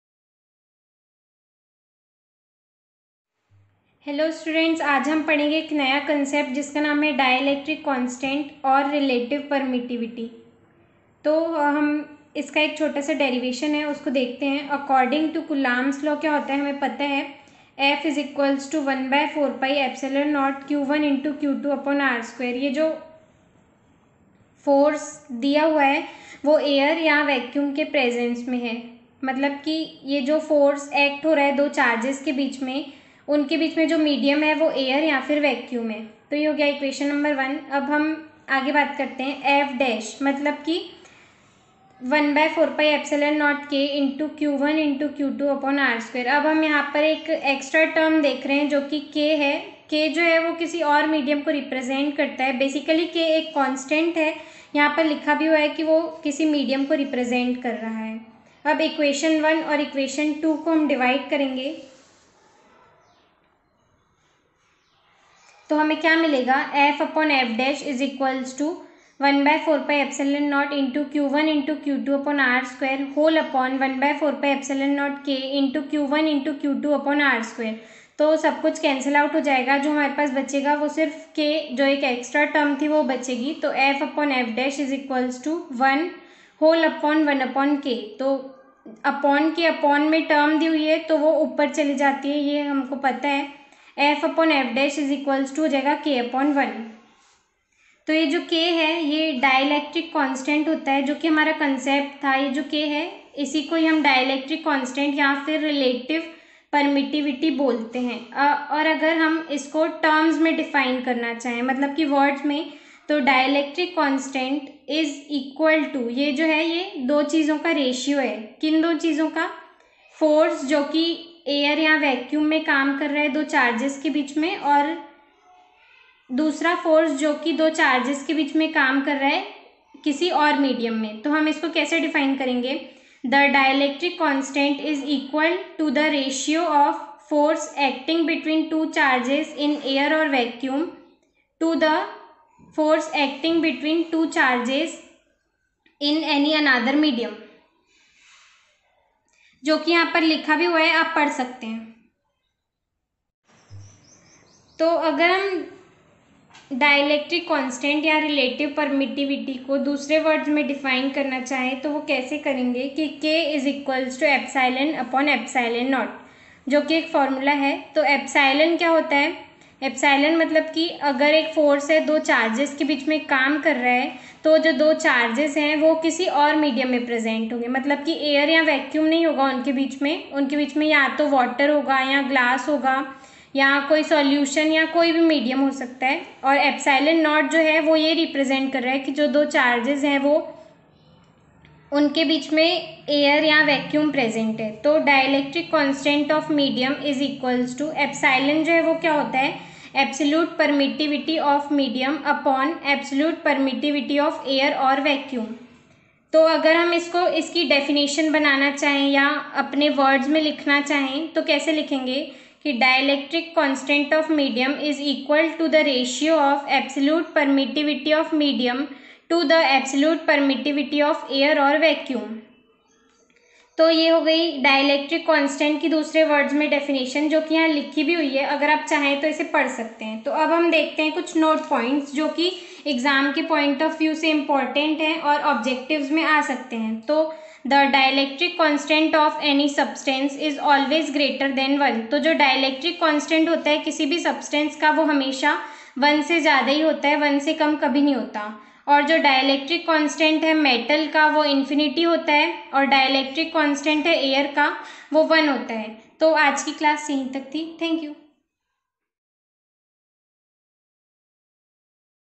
chapter 1 | electric charge and field | dielectric constant/relative permittivity | class 12 physics
Key Areas Covered in this session:
1. dielectric constant or relative permittivity – concept and derivation